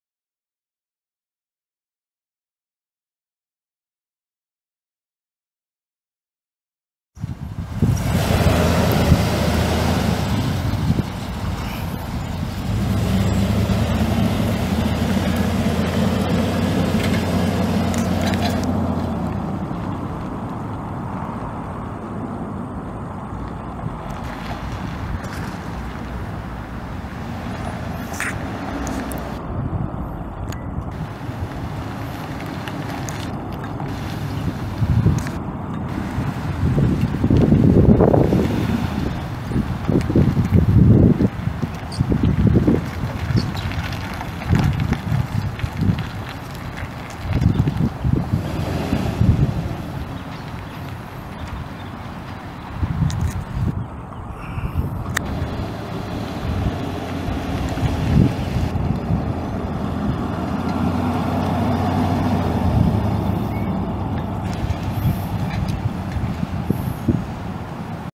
2006 Chevrolet Silverado 2500HD pickup truck for sale | no-reserve Internet auction June 20, 2017
2006 Chevrolet Silverado 2500HD pickup truck
159,045 miles on odometer
6.0L V8 OHV 16V gas engine
Automatic transmission
Tilt
Cruise
AM/FM
Power windows, locks, and mirrors
Vinyl interior
Front tow hooks
Receiver hitch
Trailer wiring
265/75R16 tires
Shifts hard
Uses oil
Oklahoma title